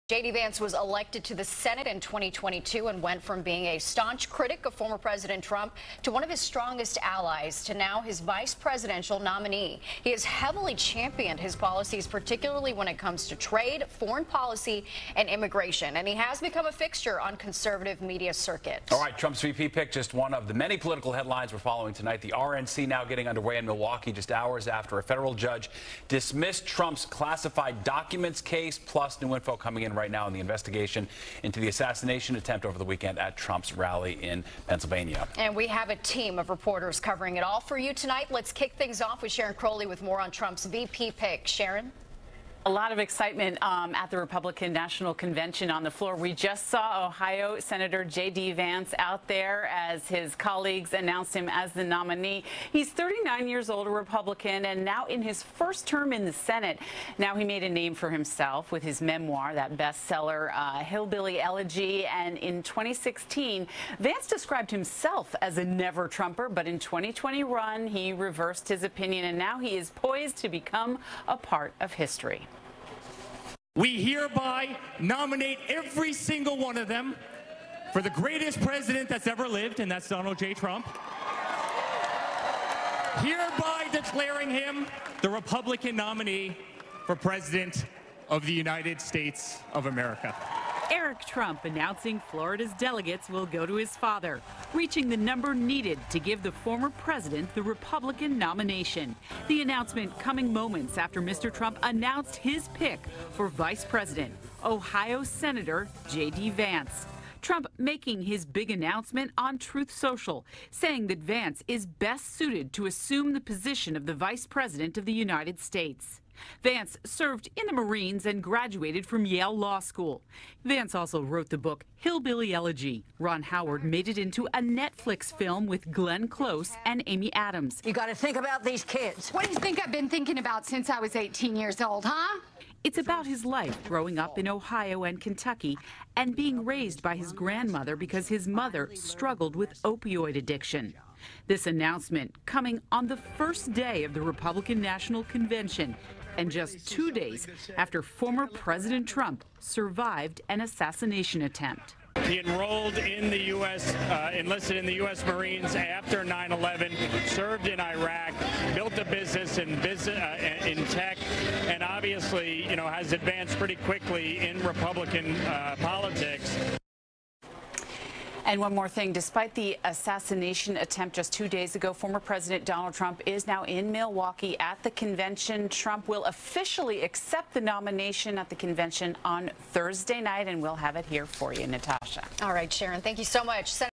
Trump chooses J.D. Vance as his VP running mate
J.D. Vance was elected to the senate in 2022 and went from being a staunch critic of former President Trump to one of his strongest allies and now his vice presidential nominee. FOX 5 NY’s Sharon Crowley reports.